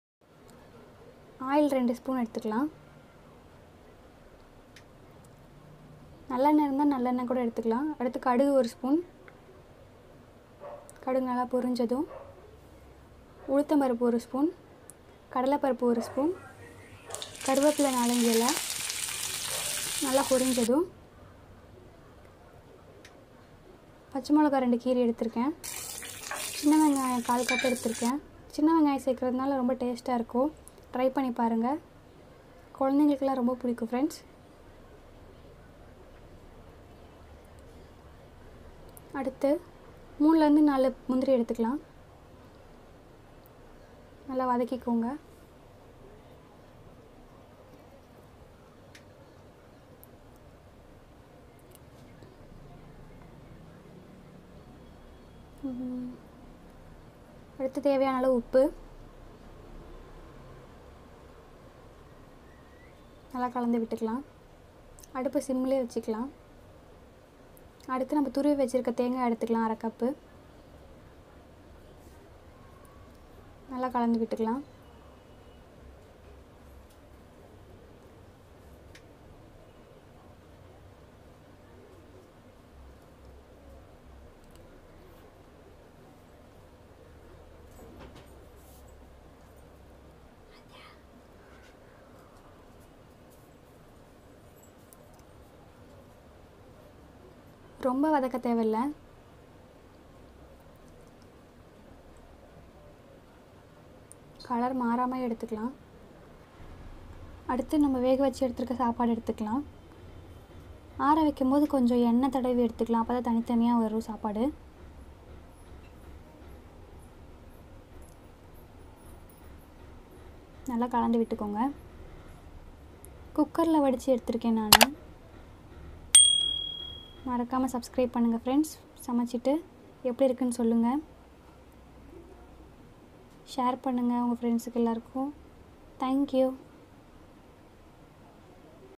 ஒரு முறை இப்படி செஞ்சி பாருங்க | thengai sadam | coconut rice | variety rice recipe | lunch box
Hello friends today's recipe is out.do check

Ingredients:
Oil_2tbs
Mustard_1tbs
Uraddhal_1tbs
Bengal gram_1tbs
Curry leaves
Green chili_2
Small onion_1/4cup
Salt_as per needed
Grated coconut_1/2cup
Boiled rice